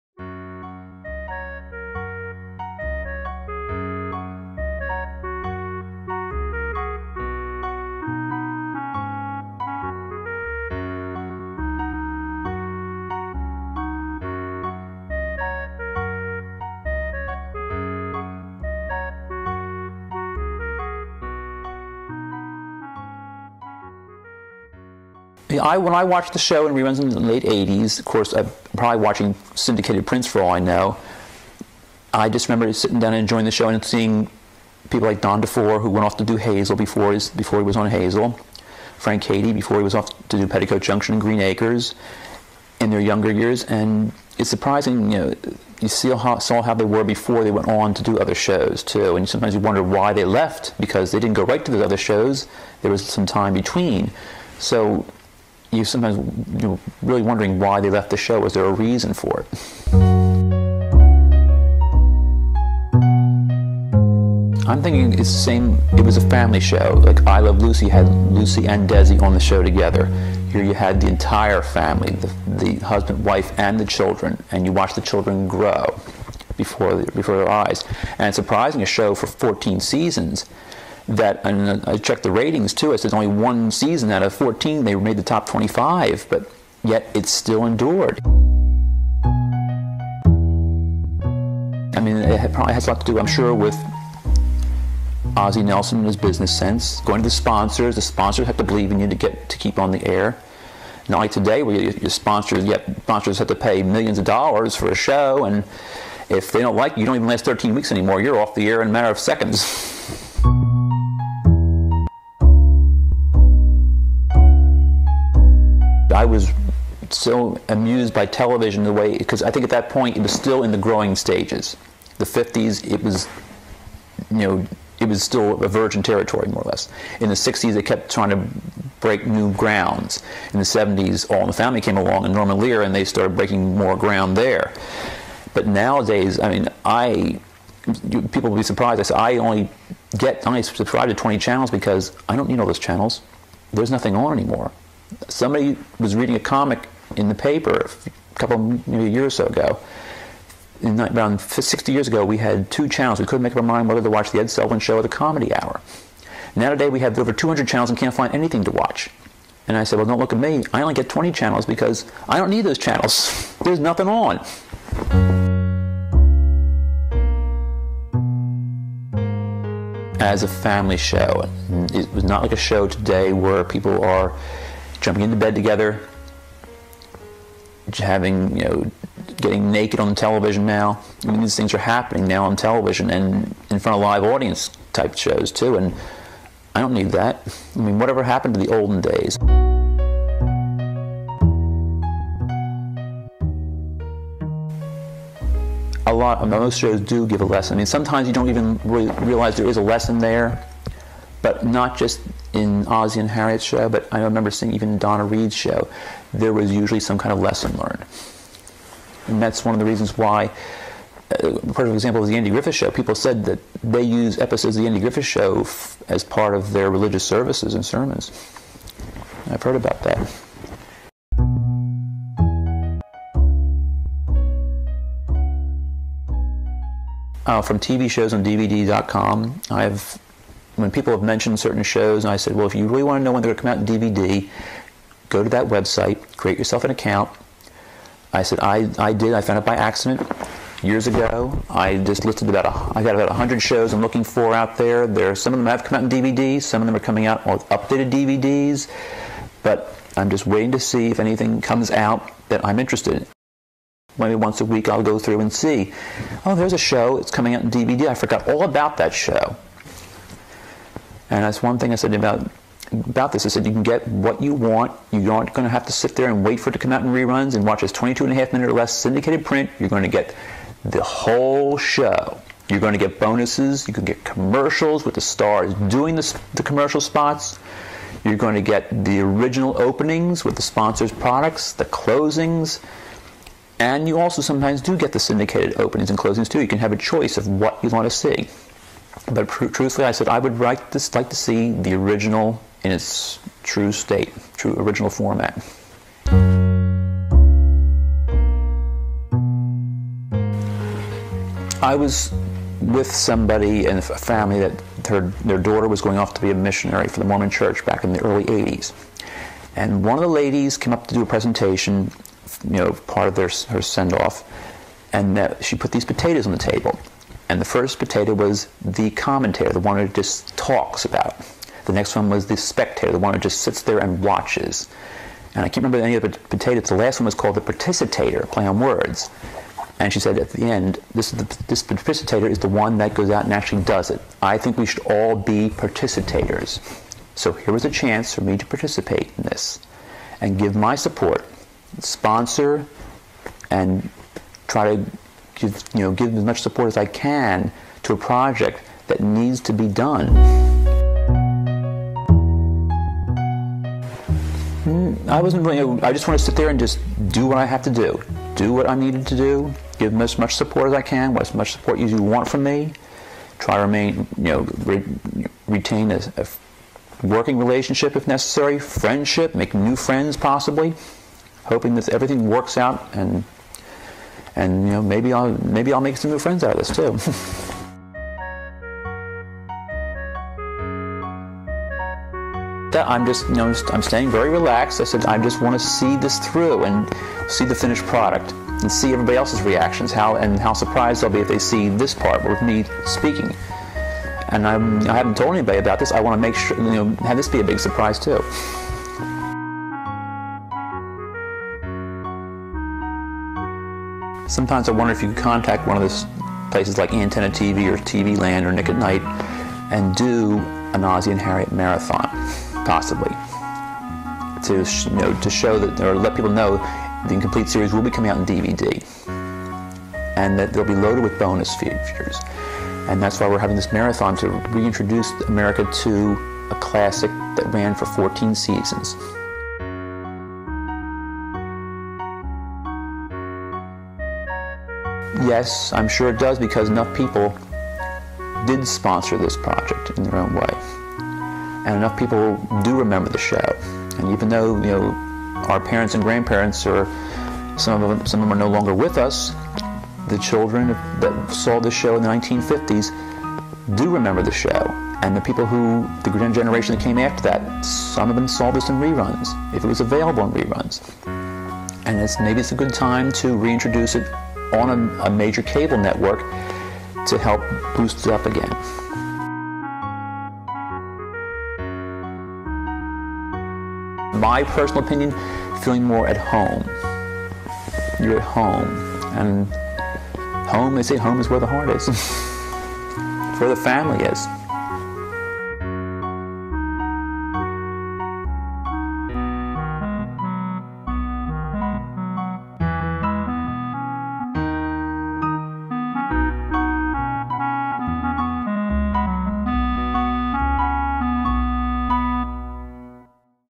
Ozzie And Harriet: James Finnegan: Donor Interview
Ozzie and Harriet: Reviving An American Treasure

A trip the through Atlantic City, New Jersey, gave me the opportunity to meet with donor James Finnegan. James had an incredible depth of television knowledge and it was a fun and informative to hear about his relationship to early television and his experience in taking part in the Ozzie and Harriet restoration project.